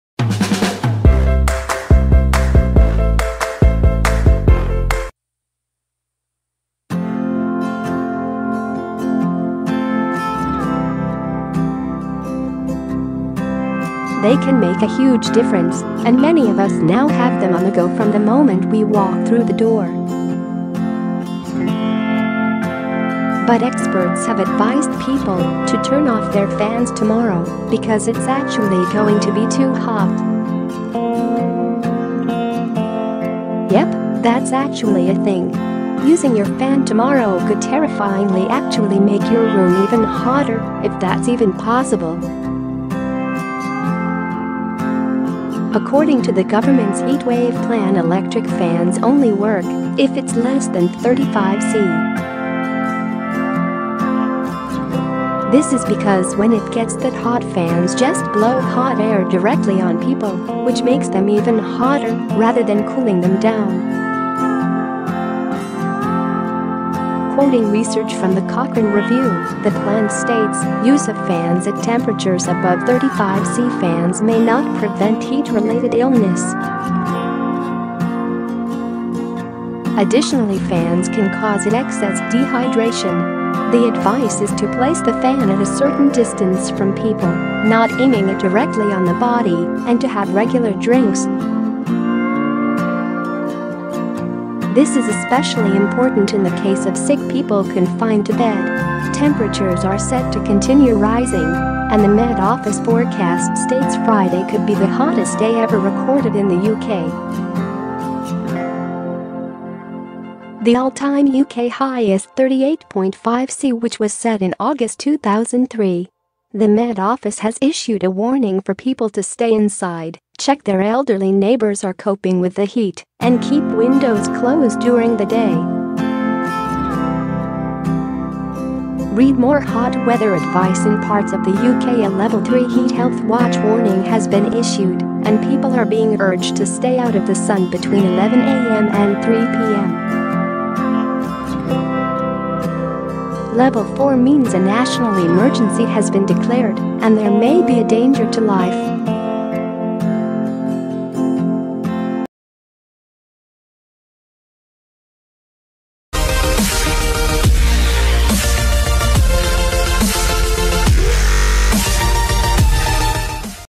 Why you shouldn't use your fan tomorrow - even though it's getting even hotter - Daily News
Heatwave /form Most of us have now given in and bought a fan in a desperate bid to get through the heatwave.  They can make a huge difference and many of us now have them on the go from the moment we walk through the door.  But experts have advised people to turn off their fans tomorrow - because it's actually going to be TOO HOT.  Yep, that's actually a thing.  Using your fan tomorrow could terrifyingly actually make your room even hotter - if that's even possible.   According to the Government's heatwave plan , electric fans only work if it's less than 35C.  This is because when it gets that hot fans just blow hot air directly on people, which makes them even hotter rather than cooling them down.  Quoting research from the Cochrane Review, the plan states: "Use of fans at temperatures above 35C fans may not prevent heat related illness.  "Additionally fans can cause excess dehydration). The advice is to place the fan at a certain distance from people, not aiming it directly on the body and to have regular drinks.   "This is especially important in the case of sick people confined to bed."  Temperatures are set to continue rising and the Met Office forecast states Friday could be the hottest day ever recorded in the UK.  The all-time UK high is 38.5C which was set in August 2003.  The Met Office has issued a warning for people to stay inside, check their elderly neighbours are coping with the heat, and keep windows closed during the day.  Read More Hot weather advice   In parts of the UK a 'level 3 heat health watch warning' has been issued and people are being urged to stay out of the sun between 11am and 3pm.  Level 4 means a national emergency has been declared and there may be a danger to life.